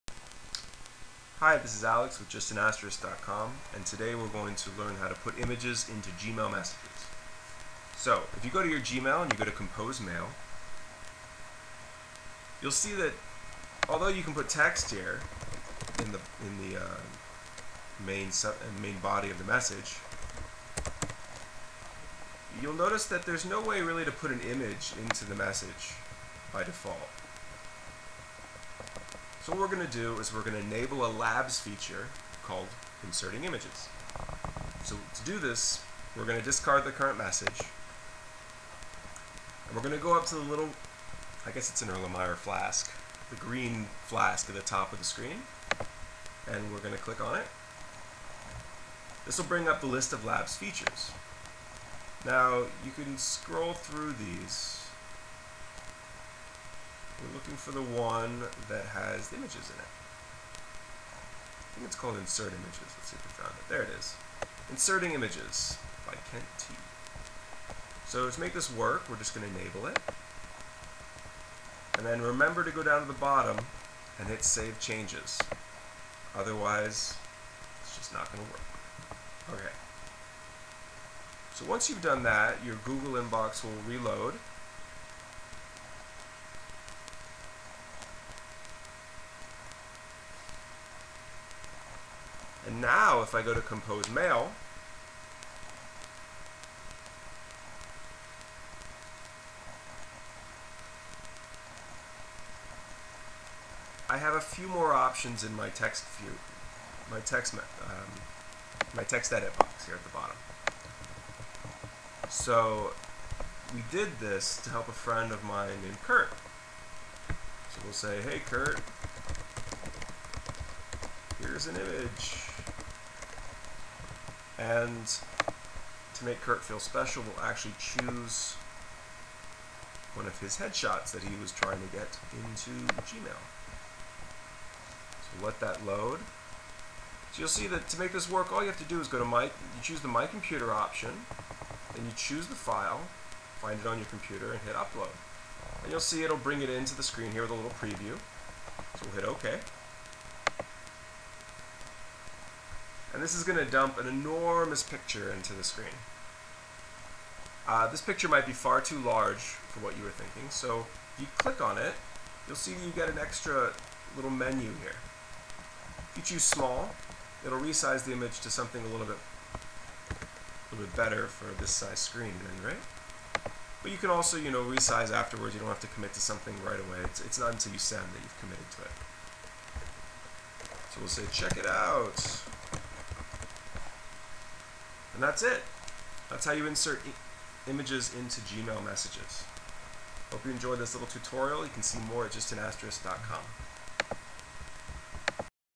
Insert pictures into Gmail emails!
It's not a hyped up feature, but with a quick "labs feature" in Gmail, you can include pictures in your email.  for more tips like this one.

~ab